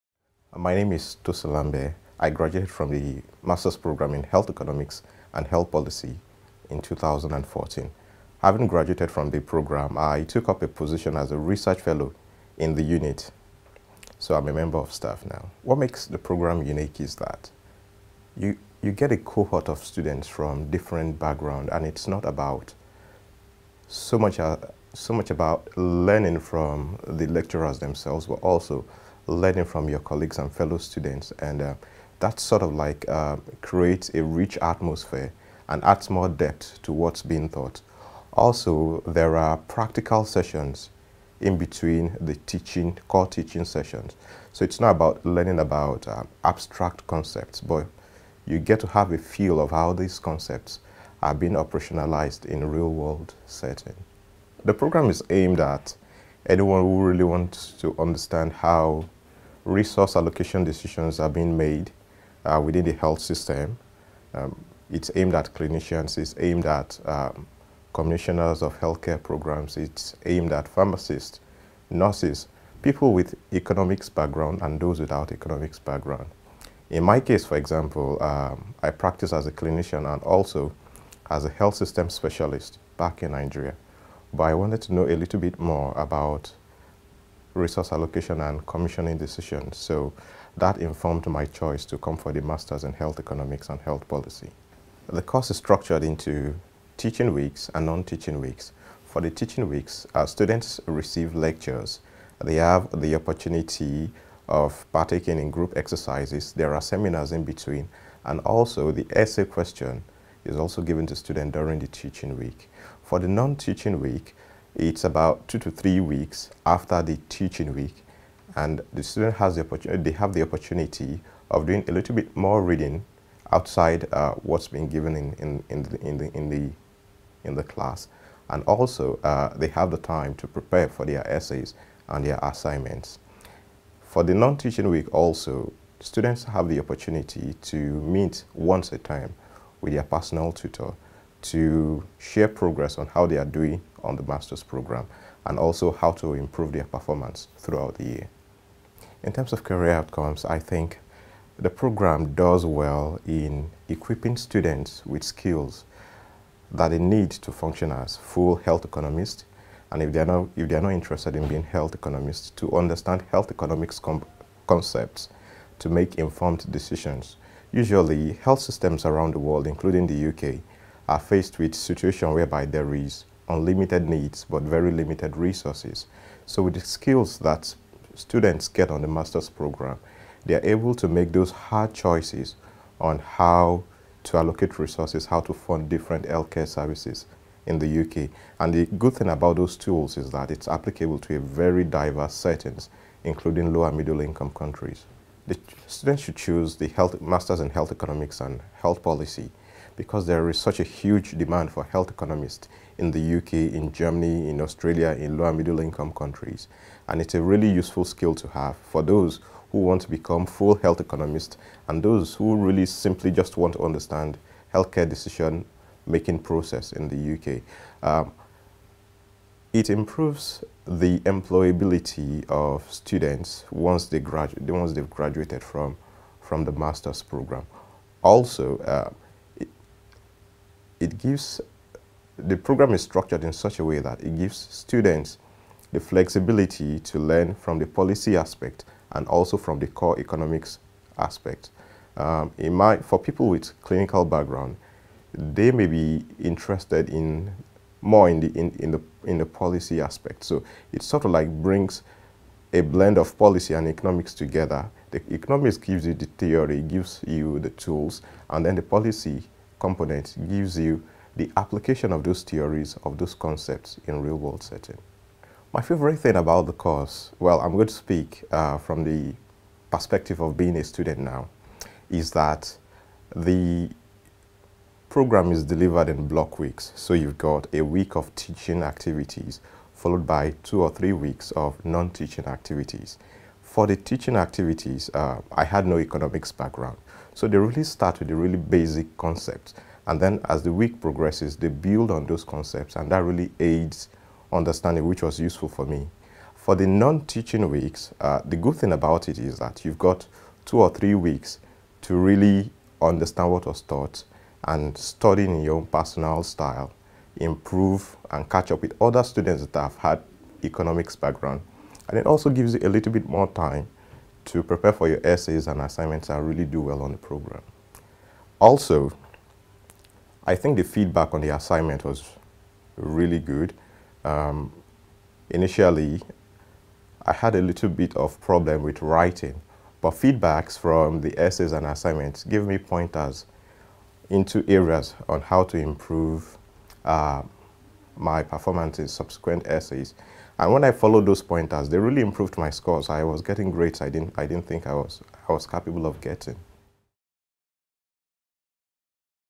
Health Economics and Health Policy - MSc/Diploma
Dr Tosin Ahmed Lambe talks about the MSc Health Economics and Econometrics programme and his journey to becomming a Research Associate.